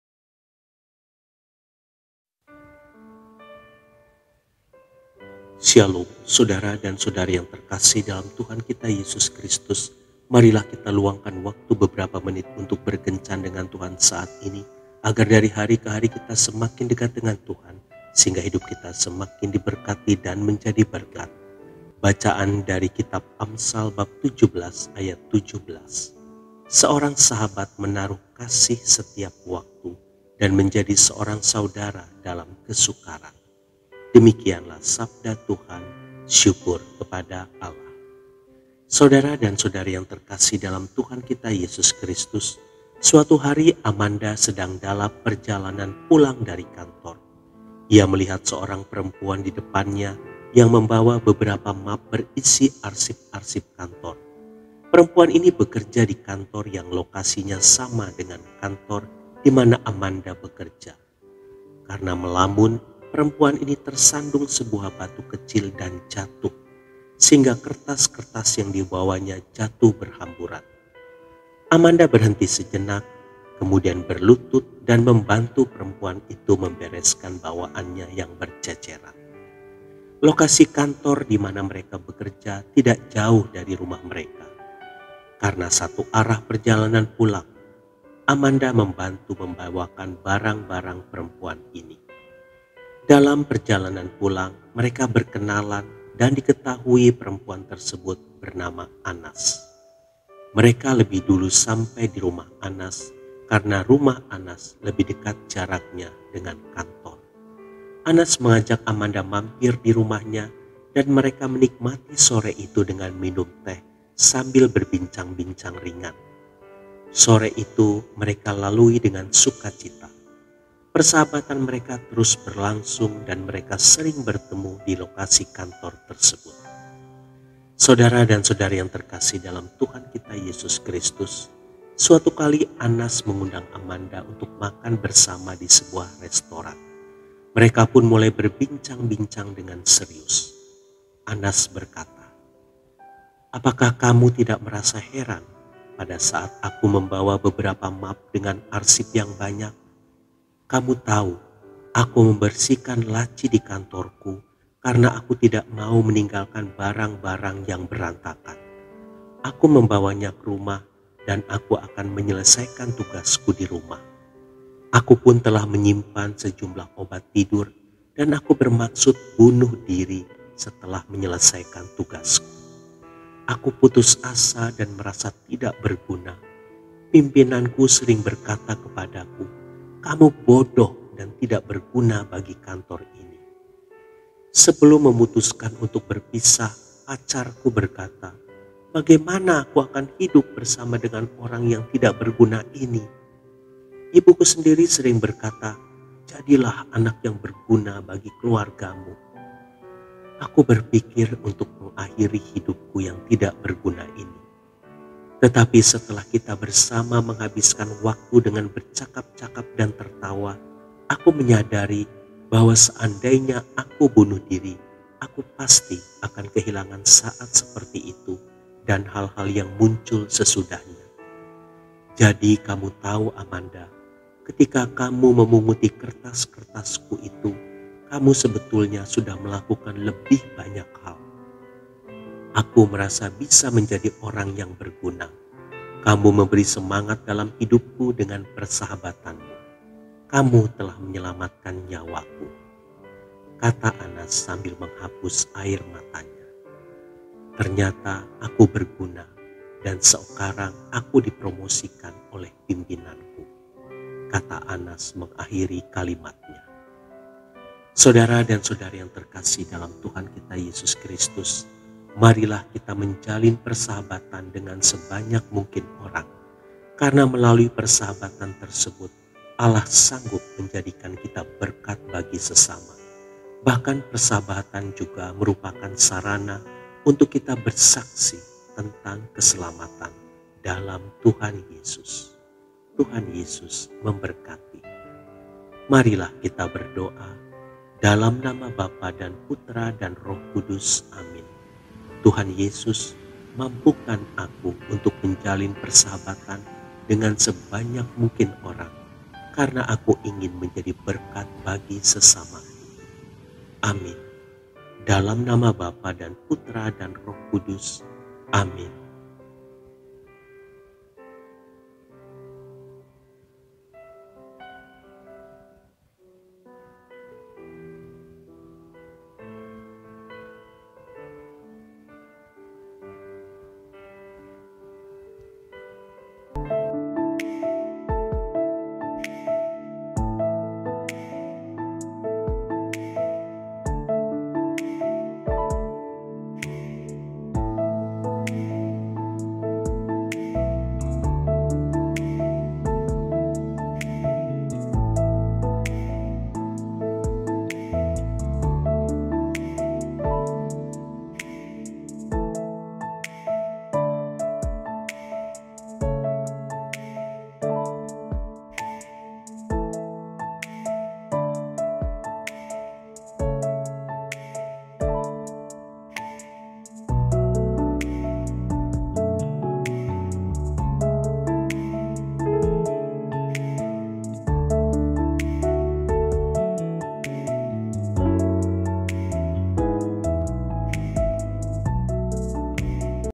Melalui persahabatan Allah sanggup menjadikan kita berkat bagi sesama.
Amsal 17:17
"Seorang sahabat menaruh kasih setiap waktu, dan menjadi seorang saudara dalam kesukaran."

Suatu hari Amanda sedang dalam perjalanan pulang dari kantor.
Dia melihat seorang perempuan di depannya yang membawa beberapa map berisi arsip-arsip kantor.

Perempuan ini bekerja di kantor yang lokasinya sama dengan kantor di mana Amanda bekerja. Karena melamun, perempuan ini tersandung sebuah batu kecil dan jatuh, sehingga kertas2 yang dibawanya jatuh berhamburan. Amanda berhenti sejenak, kemudian berlutut dan membantu perempuan itu membereskan bawaannya yang berceceran.

Lokasi kantor di mana mereka bekerja tidak jauh dari rumah mereka. Karena satu arah perjalanan pulang. Amanda membantu membawakan barang2 perempuan ini.

Dalam perjalanan pulang mereka berkenalan dan diketahui perempuan tersebut bernama Anas. Mereka lebih dulu sampai di rumah Anas, karena rumah Anas lebih dekat jaraknya dengan kantor. Anas mengajak Amanda mampir di rumahnya, dan mereka menikmati sore itu dengan minum teh sambil berbincang-bincang ringan.
Sore itu mereka lalui dengan sukacita.

Persahabatan mereka terus berlangsung dan mereka sering bertemu di lokasi kantor tersebut.

Suatu kali Anas mengundang Amanda untuk makan bersama di sebuah restoran. Mereka pun mulai berbincang-bincang dengan serius.

Anas berkata, "Apakah kamu tidak merasa heran pada saat aku membawa beberapa map dengan arsip yang banyak?
Kamu tahu, aku membersihkan laci di kantorku karena aku tidak mau meninggalkan barang2 yang berantakan.
Aku membawanya ke rumah dan akan aku selesaikan tugasku di rumah.
Aku pun telah menyimpan sejumlah obat tidur dan aku bermaksud bunuh diri setelah menyelesaikan tugasku.
Aku putus asa dan merasa tidak berguna.
Pimpinanku sering berkata kepadaku, 'Kamu bodoh dan tidak berguna bagi kantor ini.'
Sebelum memutuskan untuk berpisah, pacarku berkata, 'Bagaimana aku akan hidup bersama dengan orang yang tidak berguna ini?'
Ibuku sendiri sering berkata, "Jadilah anak yang berguna bagi keluargamu.'
Aku berpikir untuk mengakhiri hidupku yang tidak berguna ini. Tetapi setelah kita bersama menghabiskan waktu dengan bercakap-cakap dan tertawa, aku menyadari bahwa seandainya aku bunuh diri, aku pasti kehilangan saat seperti itu dan hal-hal yang muncul sesudahnya.
Jadi, kamu tahu Amanda, ketika kamu memunguti kertas-kertasku itu, kamu sebetulnya sudah melakukan lebih banyak hal.
Aku merasa bisa menjadi orang yang berguna.
Kamu memberi semangat dalam hidupku dengan persahabatanmu. Kamu telah menyelamatkan nyawaku," kata Anas sambil menghapus air matanya.

"Ternyata aku berguna dan sekarang aku dipromosikan oleh pimpinanku," kata Anas mengakhiri kalimatnya.

Marilah kita menjalin persahabatan dengan sebanyak mungkin orang, karena melalui  persahabatan tersebut Allah sanggup menjadikan kita berkat bagi sesama. Bahkan persahabatan juga merupakan sarana untuk kita bersaksi tentang keselamatan dalam Tuhan Yesus.

Doa:
Tuhan Yesus, mampukan aku untuk menjalin persahabatan dengan sebanyak mungkin orang, karena aku ingin menjadi berkat bagi sesamaku. Amin. (Dod).